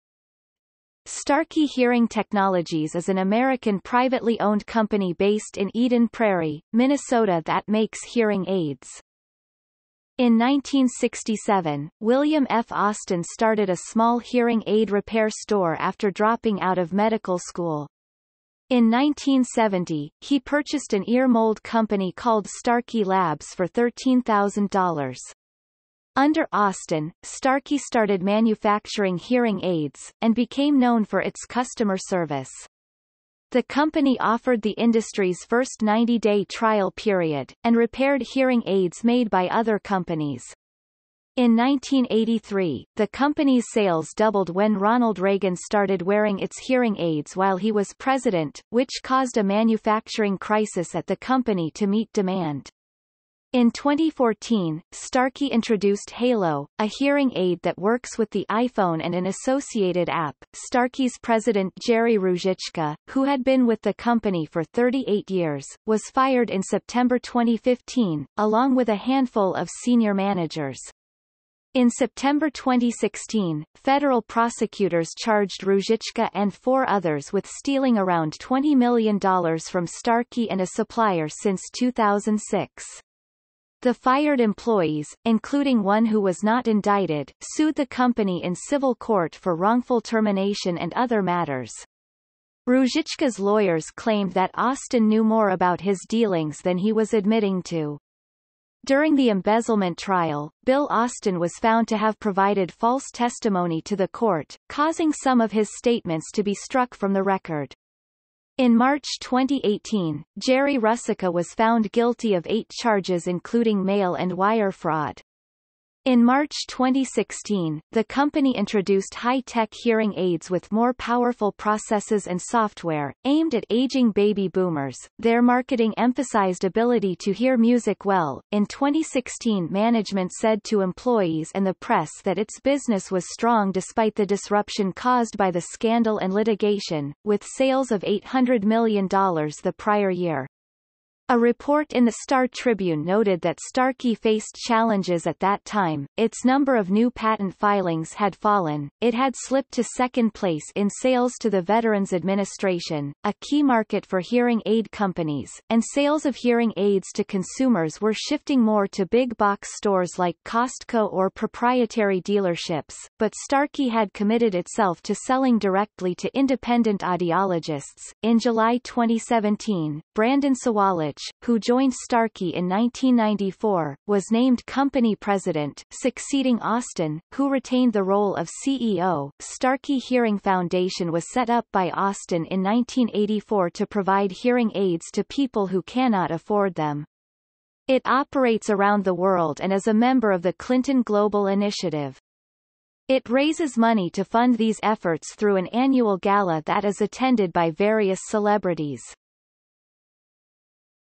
Starkey Laboratories | Wikipedia audio article
- Socrates

SUMMARY
Starkey Hearing Technologies is an American privately owned company based in Eden Prairie, Minnesota that makes hearing aids.
In 1967, William F. Austin started a small hearing aid repair store after dropping out of medical school. In 1970, he purchased an ear mold company called Starkey Labs for $13,000.  Under Austin, Starkey started manufacturing hearing aids, and became known for its customer service. The company offered the industry's first 90-day trial period, and repaired hearing aids made by other companies. In 1983, the company's sales doubled when Ronald Reagan started wearing its hearing aids while he was president, which caused a manufacturing crisis at the company to meet demand. In 2014, Starkey introduced Halo, a hearing aid that works with the iPhone and an associated app.Starkey's president Jerry Ruzicka, who had been with the company for 38 years, was fired in September 2015, along with a handful of senior managers. In September 2016, federal prosecutors charged Ruzicka and four others with stealing around 20 million dollars from Starkey and a supplier since 2006. The fired employees, including one who was not indicted, sued the company in civil court for wrongful termination and other matters. Ruzicka's lawyers claimed that Austin knew more about his dealings than he was admitting to. During the embezzlement trial, Bill Austin was found to have provided false testimony to the court, causing some of his statements to be struck from the record. In March 2018, Jerry Rusicka was found guilty of 8 charges including mail and wire fraud. In March 2016, the company introduced high-tech hearing aids with more powerful processes and software, aimed at aging baby boomers; their marketing emphasized ability to hear music well.In 2016 management said to employees and the press that its business was strong despite the disruption caused by the scandal and litigation, with sales of $800 million the prior year. A report in the Star Tribune noted that Starkey faced challenges at that time; its number of new patent filings had fallen, it had slipped to second place in sales to the Veteran's Administration, a key market for hearing aid companies, and sales of hearing aids to consumers were shifting more to big box stores like Costco or proprietary dealerships, but Starkey had committed itself to selling directly to independent audiologists.In July 2017, Brandon Sawalich, who joined Starkey in 1994, was named company president, succeeding Austin, who retained the role of CEO.Starkey Hearing Foundation was set up by Austin in 1984 to provide hearing aids to people who cannot afford them. It operates around the world and is a member of the Clinton Global Initiative. It raises money to fund these efforts through an annual gala that is attended by various celebrities.